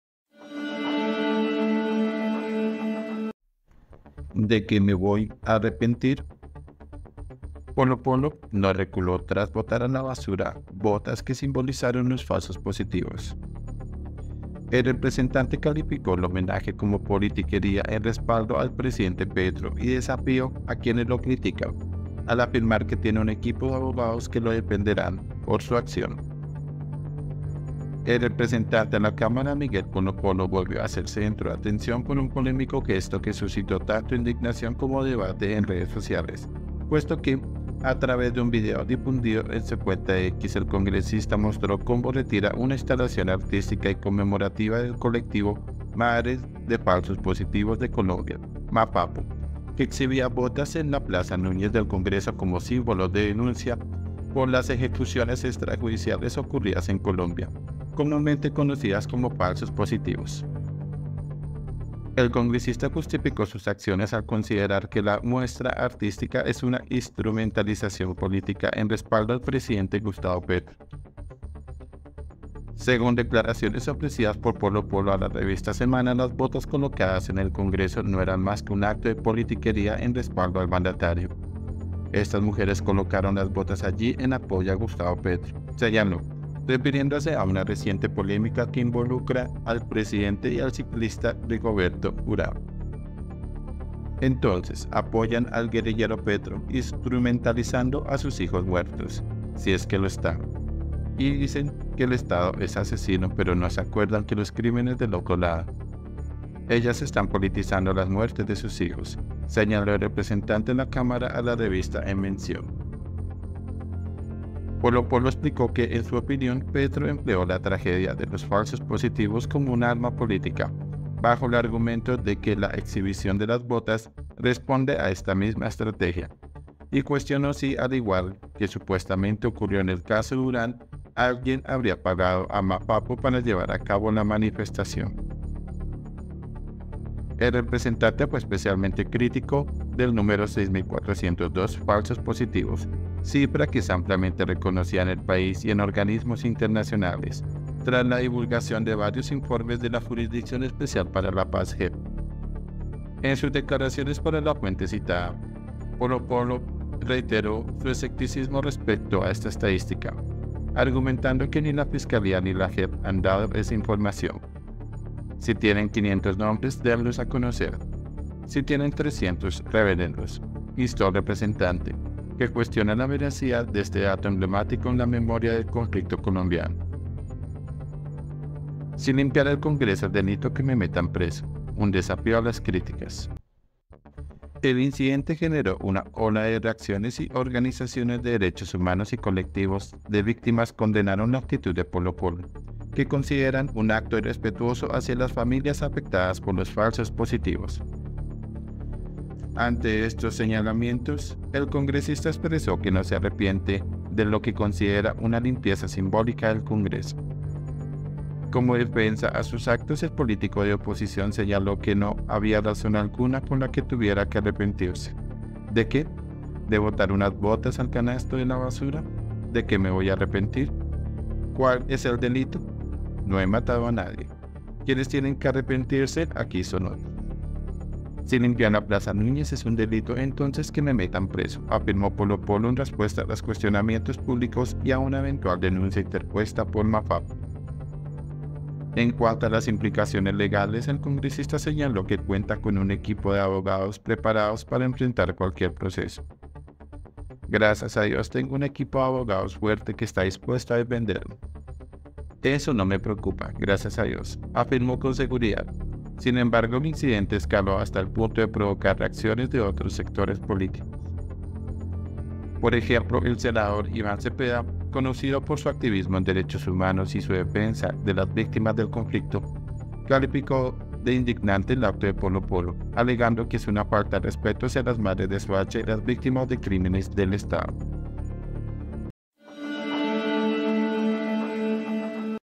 “¿DE QUÉ ME VOY A ARREPENTIR?” ⛔ POLO POLO DESAFÍA LAS DURAS CRÍTICAS
El representante calificó el homenaje como “politiquería” en respaldo al presidente Petro y desafió a quienes lo critican, al afirmar que tiene un equipo de abogados que lo defenderán por su acción
INFOBAE